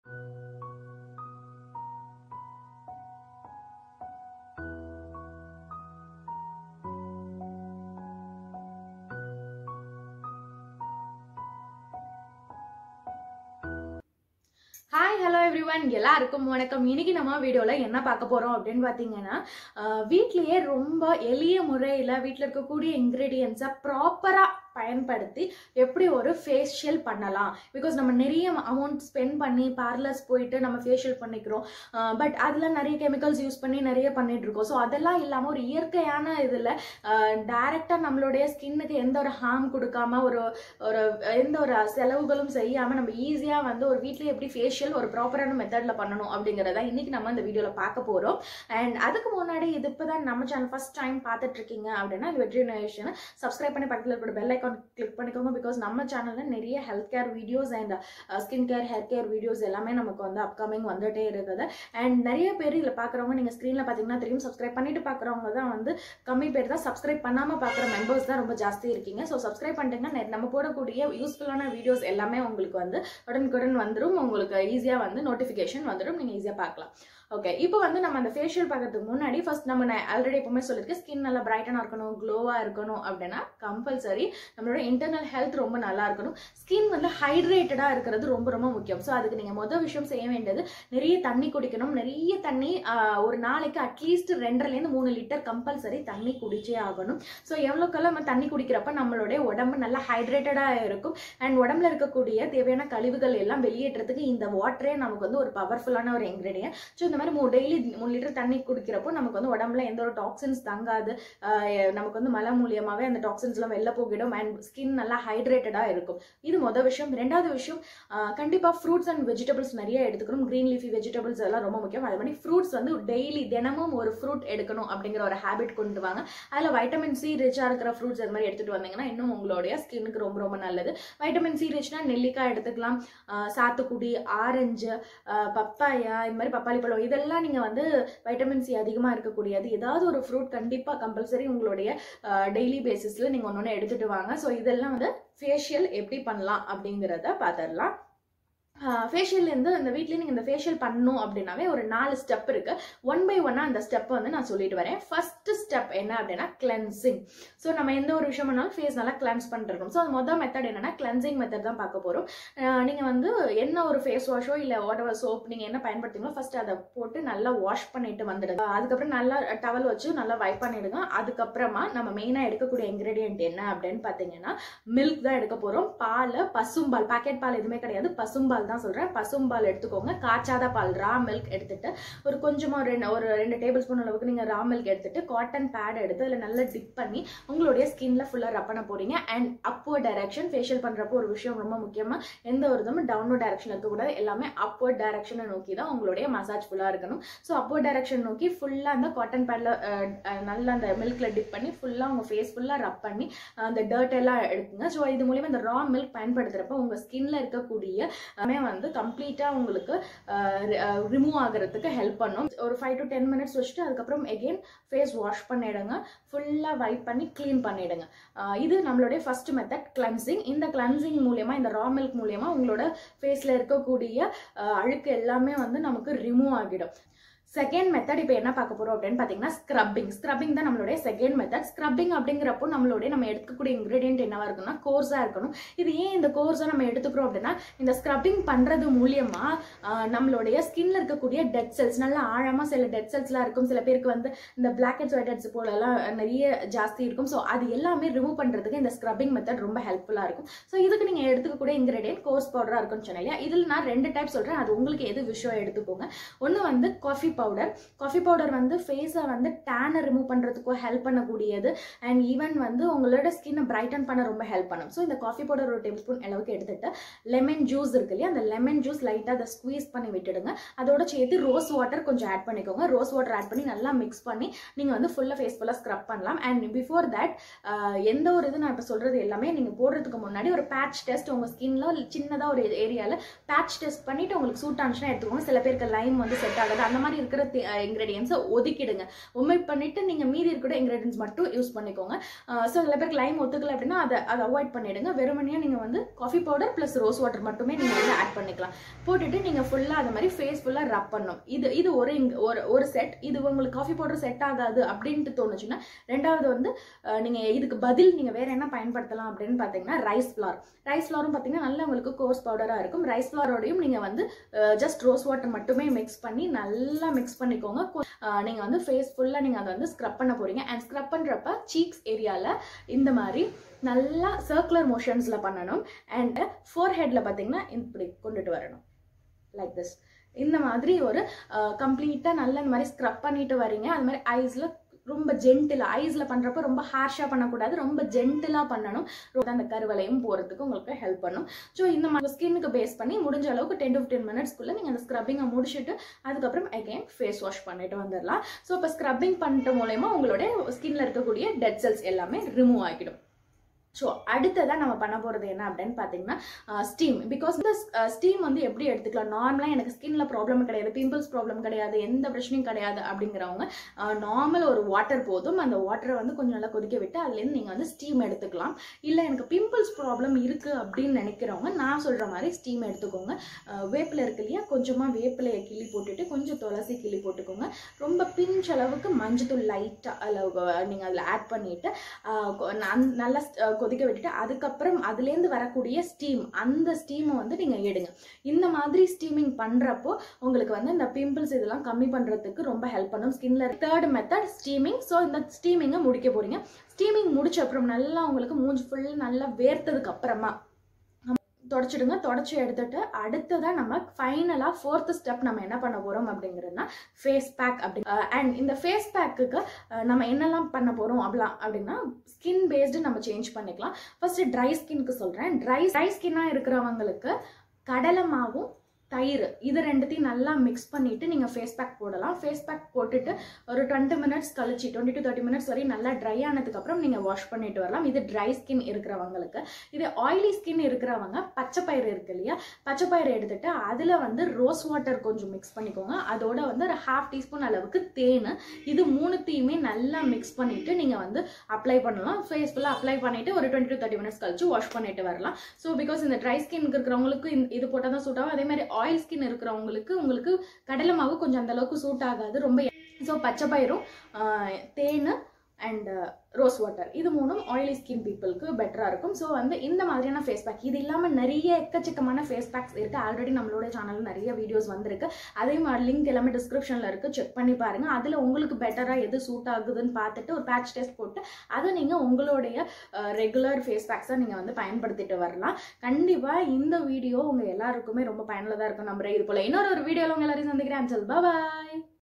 முகம் வெள்ளையாக Beauty parlour secret | skin brightening facial at home
This video about skin brightening  home facial revealing beauty parlour secret it helps to reduce pigmentation pimple marks and gives skin whitening skin glowing explained in tamil